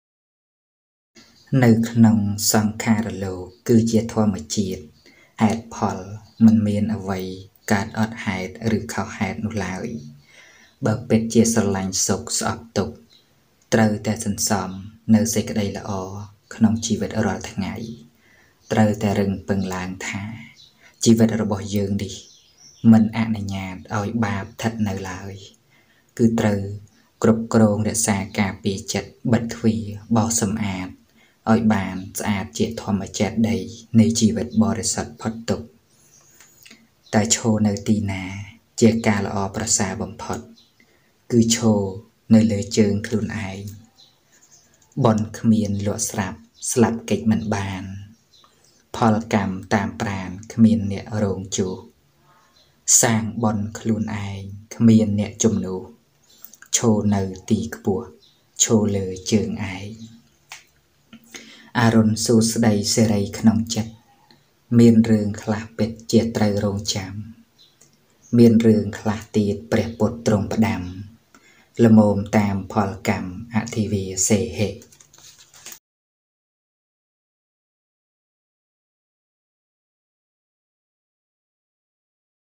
នៅក្នុងសង្ខារលោក ,Dhamma for mind educaion
និយាយពីការអប់រំចិត្តតាមព្រះពុទ្ធដីការ